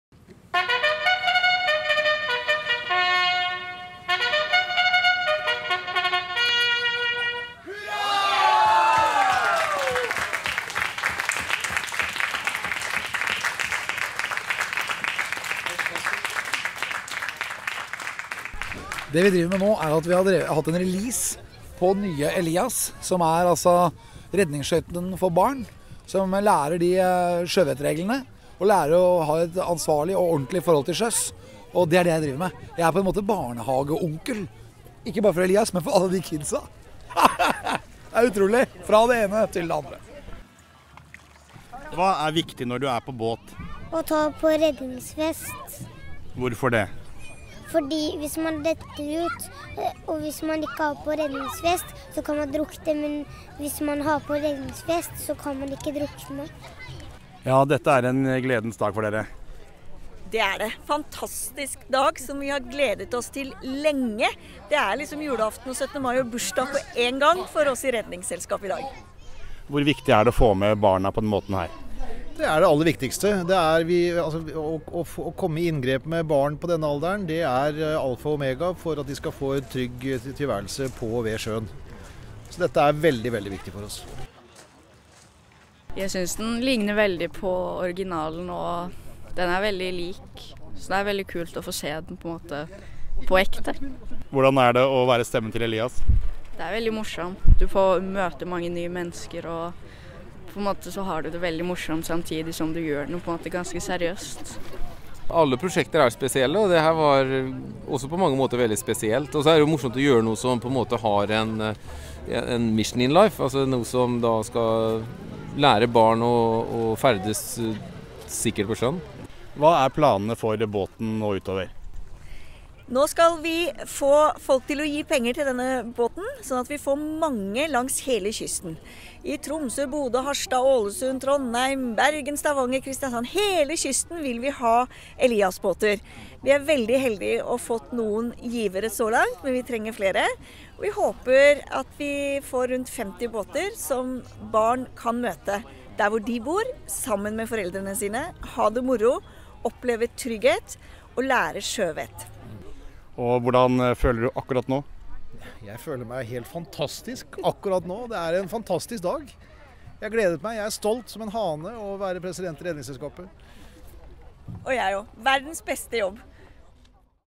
Redningsskøyta Elias sjøsettes
I mars ble Redningsselskapets nye Elias-båt sjøsatt og prøvekjørt av Alex Rosén. Nye Elias er designet av Bård Eker, og sommeren 2014 blir den å se langs hele kysten vår. Følg med på eliasklubben.no for å finne ut hvor du kan møte den!